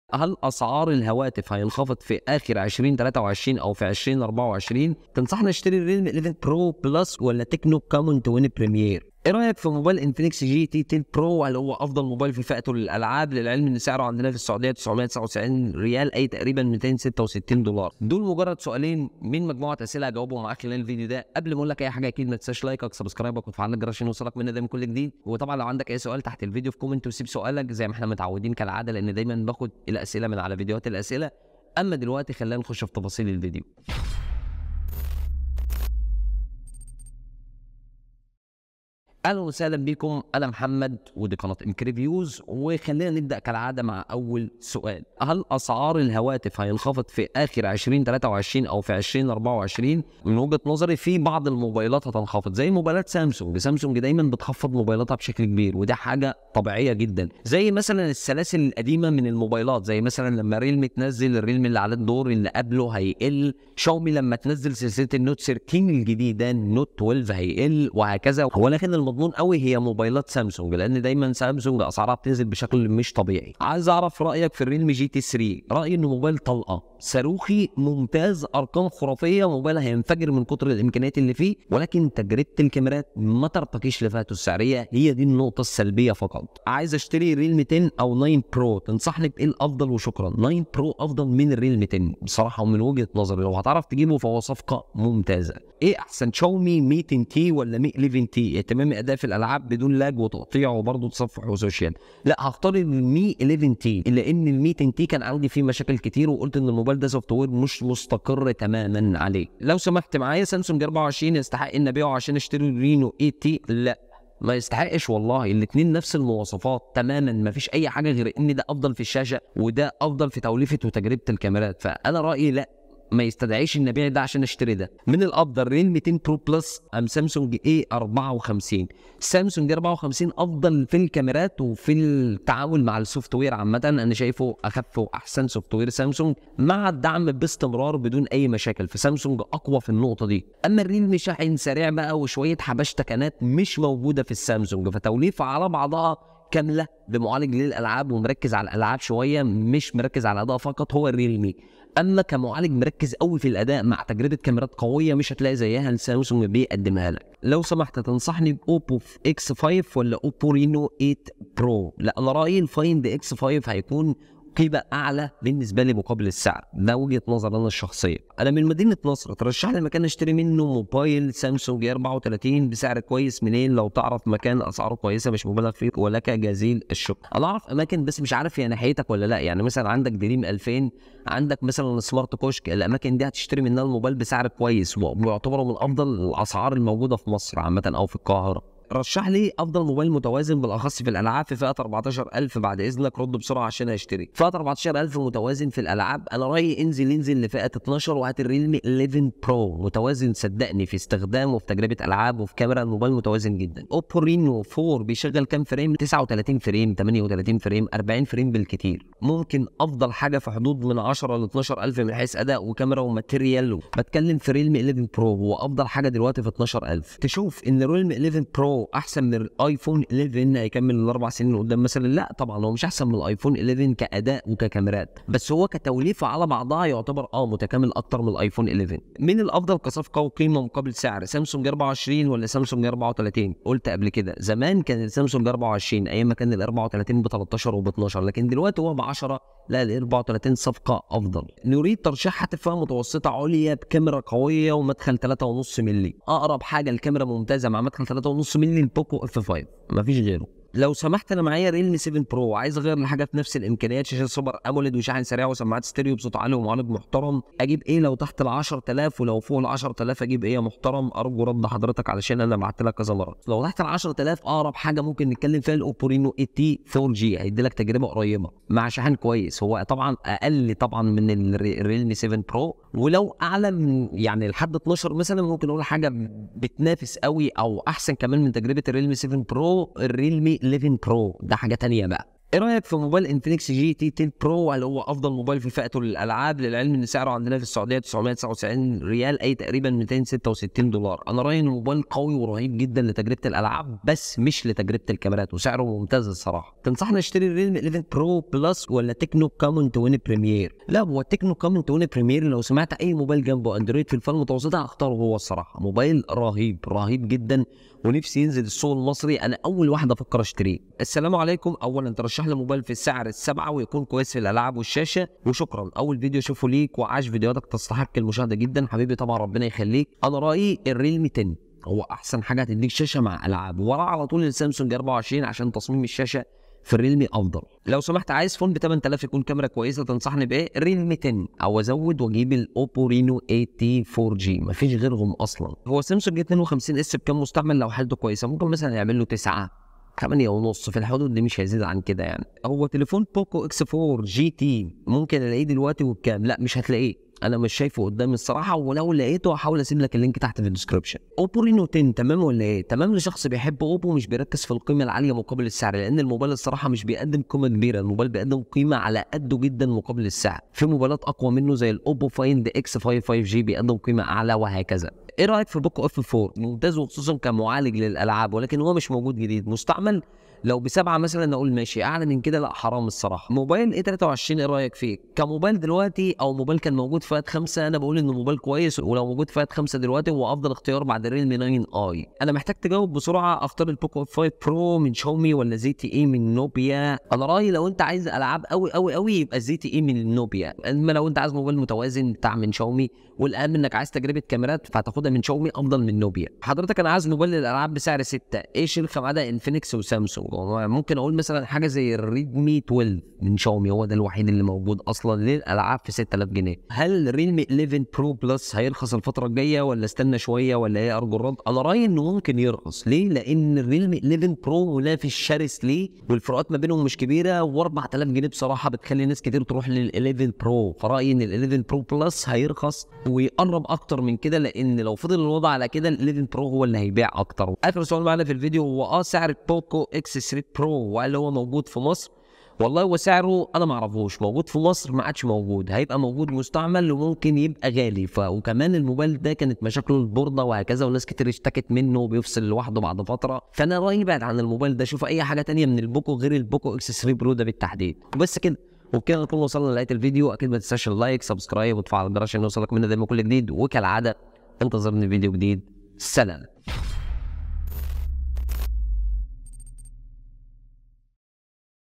حرفيا ده اكتر موبايل بيقدم قيمة مقابل السعر بدون منافس | واخيرا اسعار الموبايلات هتنزل نهاية 2023 !
اكتر موبايل حبيته خلال السنة كلها 2023
افضل موبايل على الاطلاق بيقدم قيمة مقابل سعر
تكنو كامون 20 بريميير Techno Camon 20 Premiere
هل اسعار الموبايلات هتنزل نهاية 2023 وبداية 2024 ؟
raelme 11 pro plus ولا سامسونج a54 ؟
Oppo find x5 ولا Oppo reno 8 pro ؟
افضل موبايل متوازن وخصوصا فى الالعاب بسعر 14000 جنيه ؟
realme 11 pro ولا iPhone 11 ؟

دول مجرد اسئلة من اسئلتكم الرايقة ان شاء الله هجاوب عليها خلال الفيديو
متنساش لو عندك اى سؤال تحت الفيديو فى كومنت سيب سؤالك
واكيد طبعا متنساش تدعمنى باللايك والاشتراك الرايق جدا